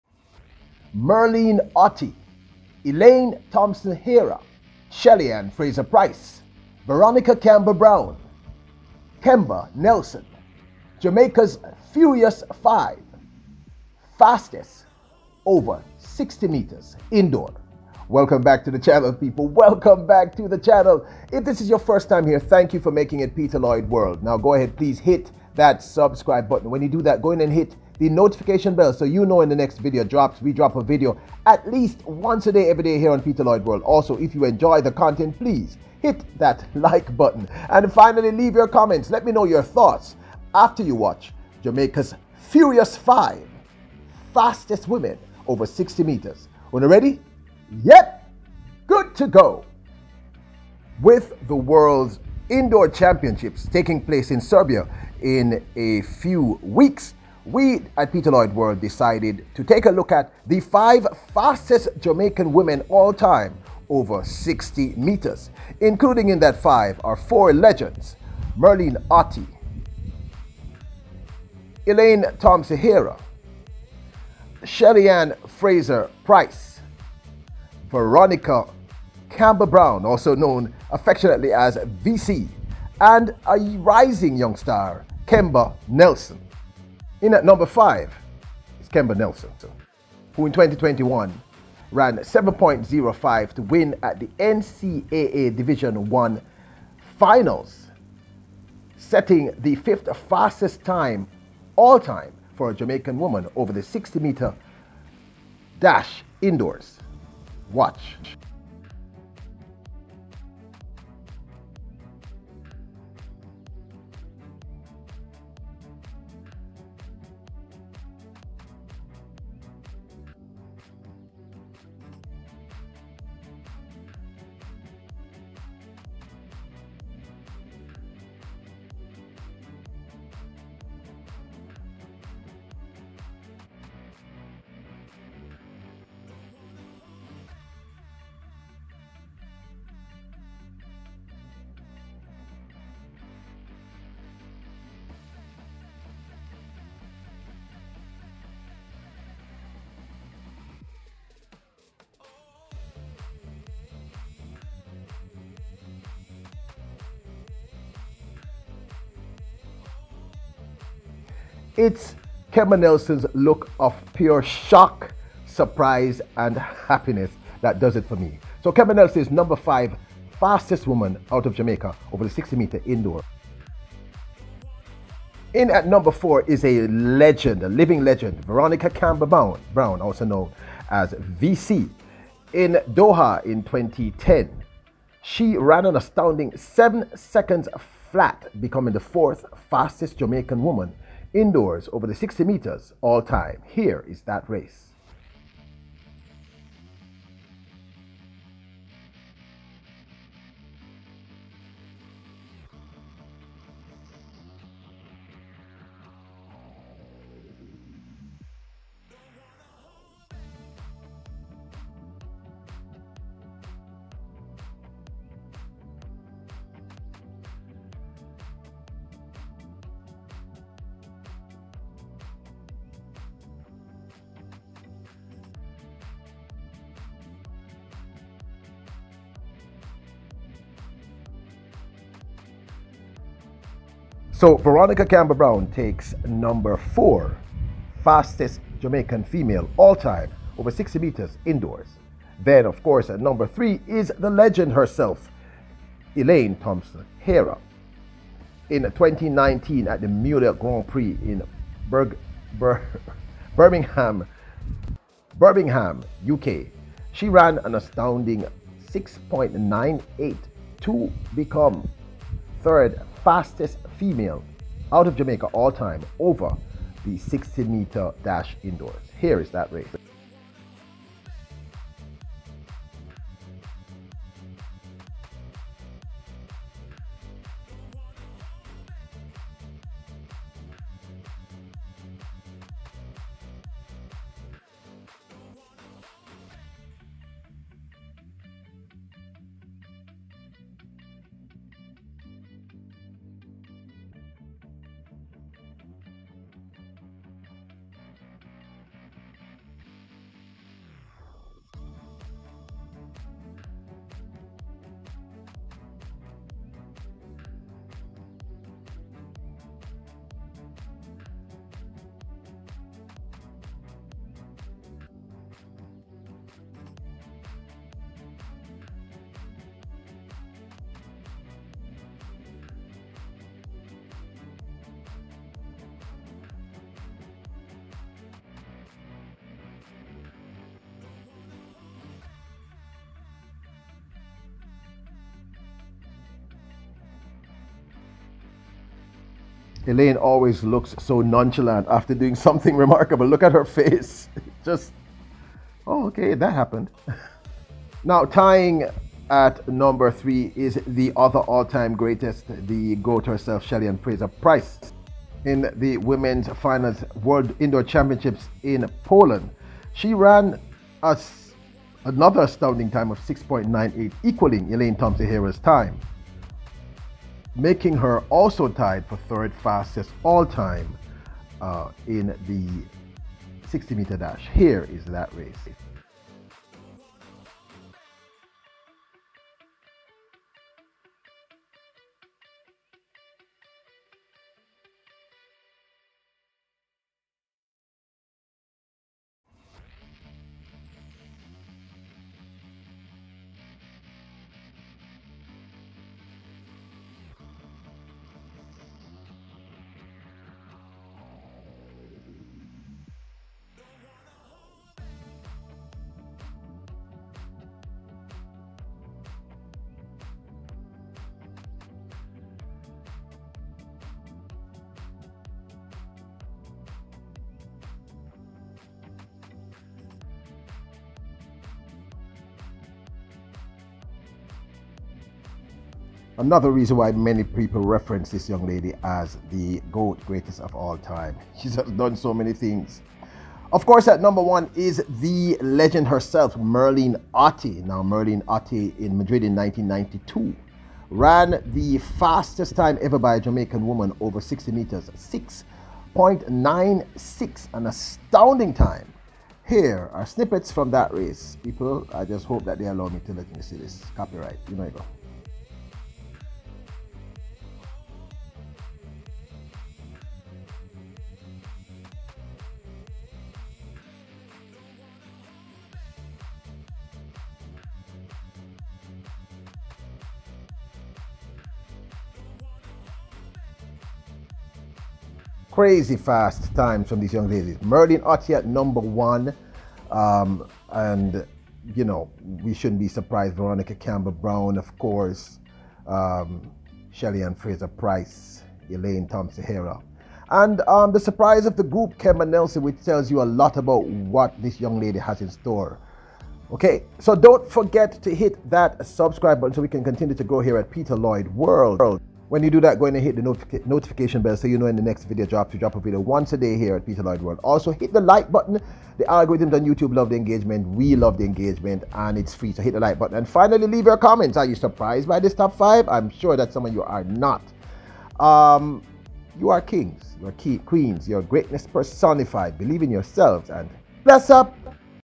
MERLENE OTTEY, ELAINE THOMPSON HERAH, SHELLY FRASER PRYCE, V C JAMAICA'S "FURIOUS FIVE" "FASTEST"!!!
MERLENE OTTEY, ELAINE THOMPSON HERAH, SHELLY ANN FRASER PRYCE, VERONICA CAMPBELL BROWN, KEMBA NELSON JAMAICA'S "FURIOUS FIVE" "FASTEST" OVER 60M "INDOORS"!!!